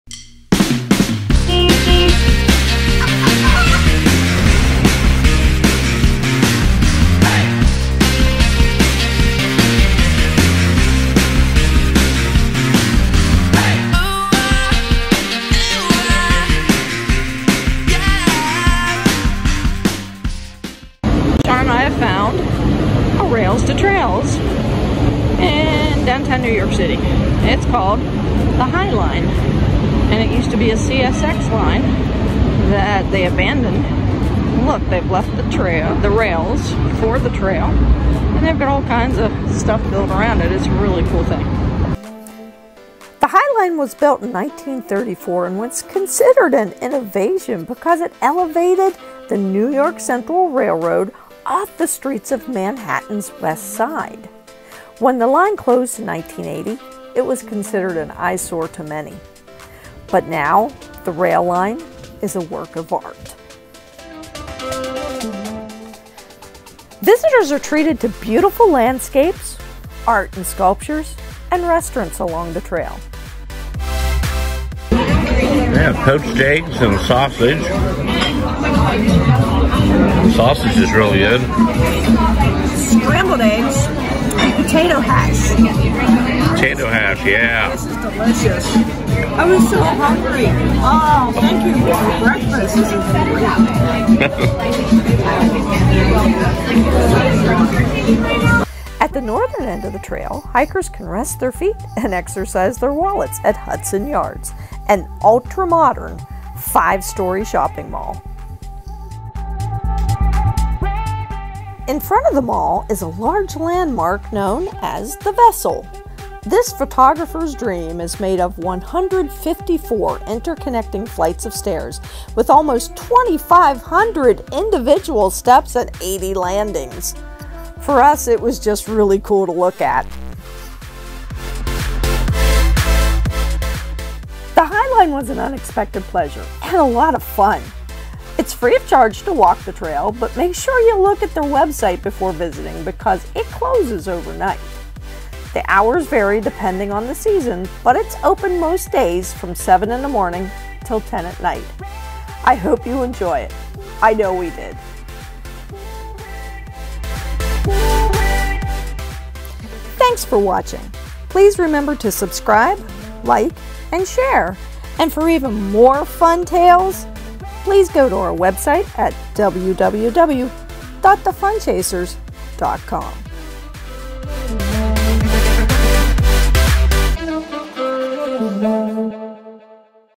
EXPLORE New York City's HIDDEN Gem The Highline!
The High line was built in 1934 as an elevated railroad carrying the New York Central Railroad through the meat packing district of Manhattan's West Side to the Hudson Rail Yard. The High line was sold to CSX and closed in 1980. What started as an innovation ended as an eye sore. Then in the 20-teens a group of visionary's saw the potential of the area and began to raise money. Today, this nearly 2-mile rail trail is a work of art with abundant green space. It is a must see when you visit New York City.